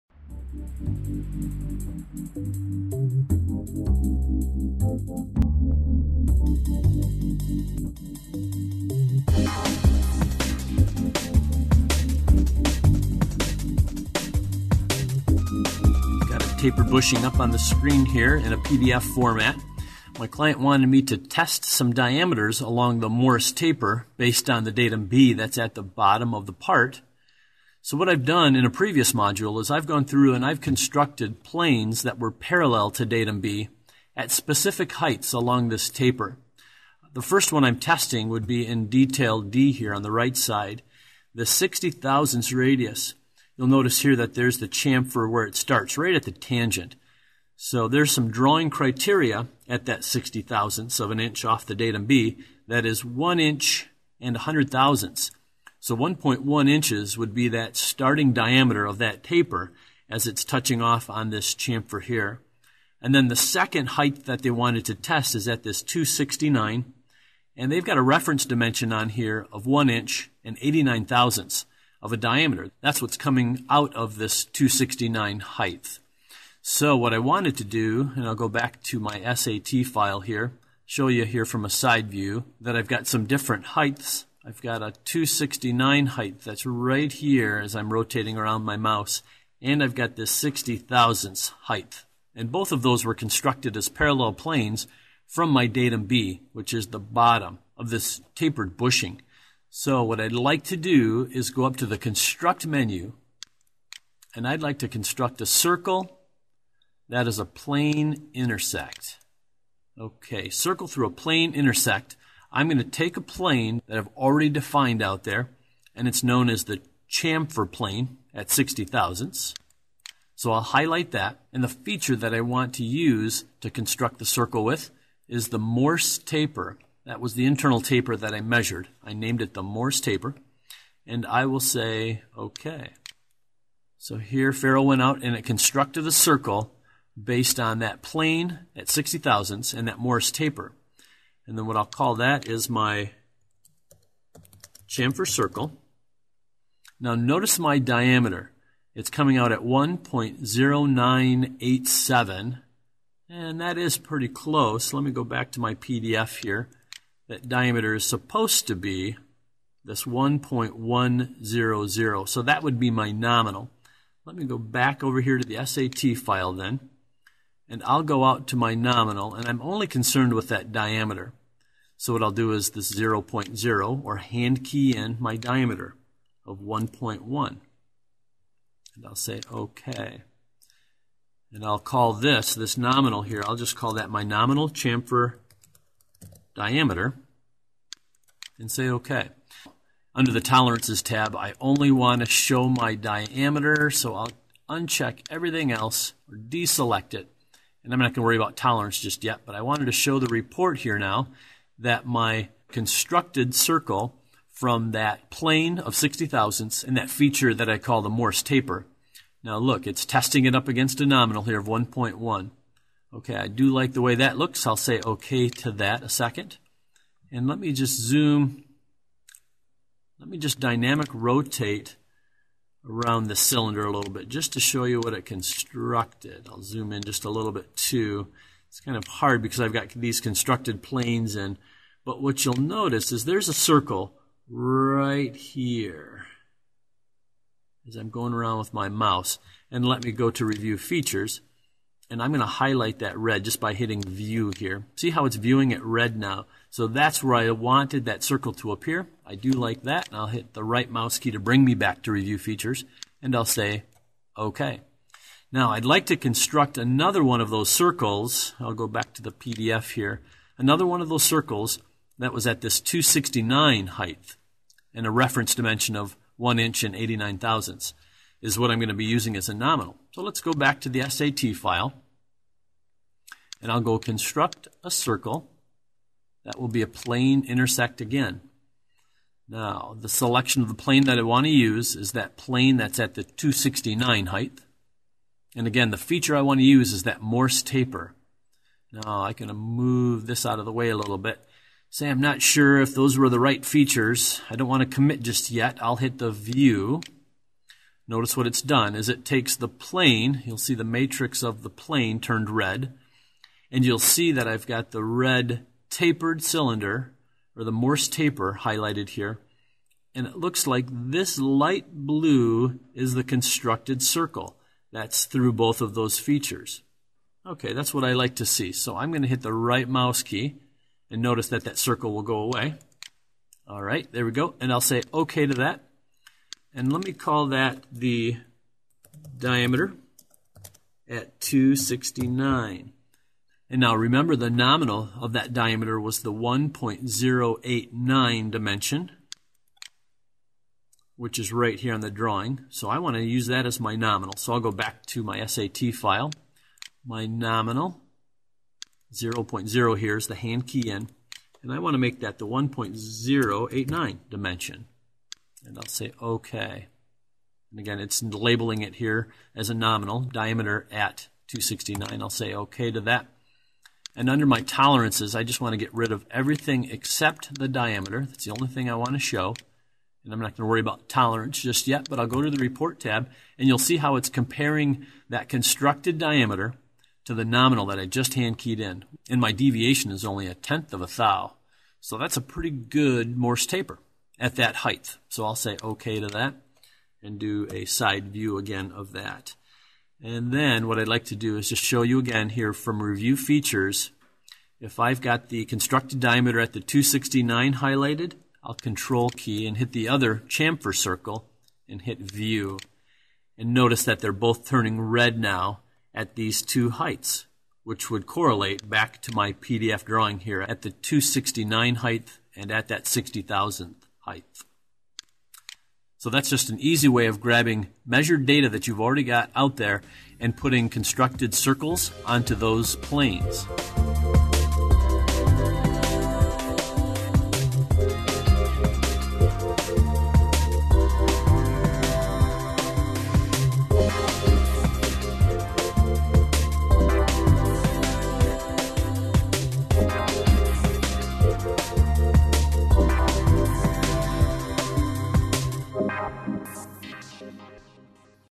Faro Arm Training CAM2: Circle - Plane Intersect (4.0 & X)
This CAM2 Measure training video will explain how to take a cylinder and intersect it through a previously measured plane and use a constructed to retrieve information. This is useful for retrieving information from measured features in a measurement session.